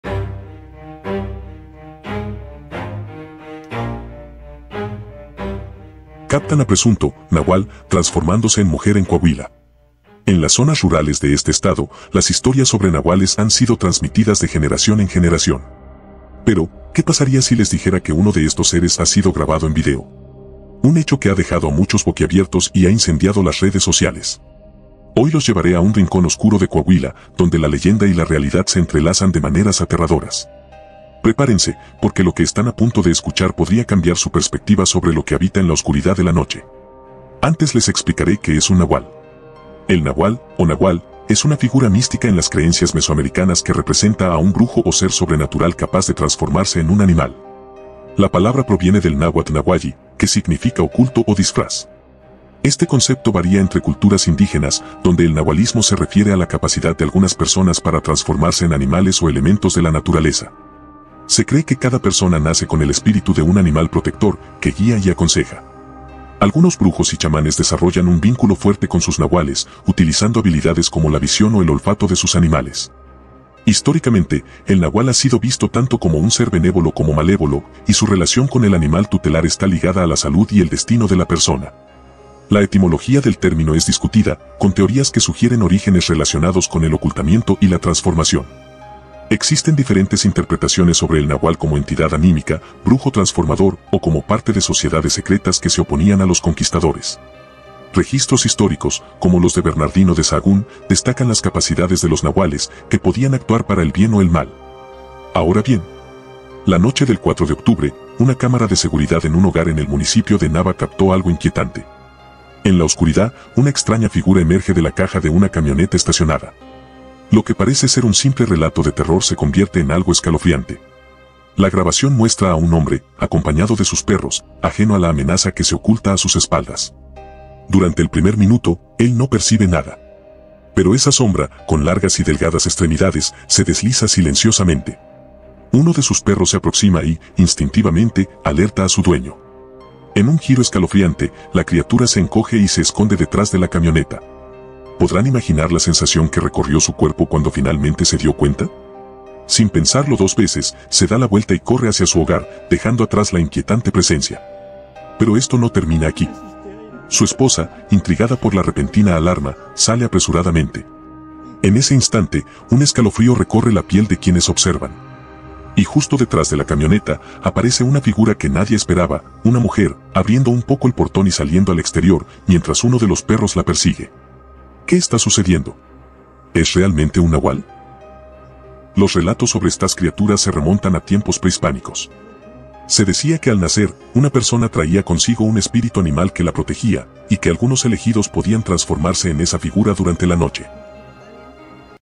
Captan a Presunto NAHUAL Transformándose en Mujer
En este escalofriante episodio, exploramos el inquietante fenómeno de un presunto nahual captado en video transformándose en mujer. Acompáñanos mientras desentrañamos la leyenda detrás de estas misteriosas criaturas que han fascinado a generaciones.

🕛 La grabación, tomada en plena noche, revela momentos aterradores y sorprendentes. ¿Es real o un simple truco de la cámara? Analizaremos la historia, el significado de los nahuales y testimonios de quienes han vivido encuentros cercanos.

¡Déjanos tus comentarios y cuéntanos qué piensas sobre esta inquietante transformación!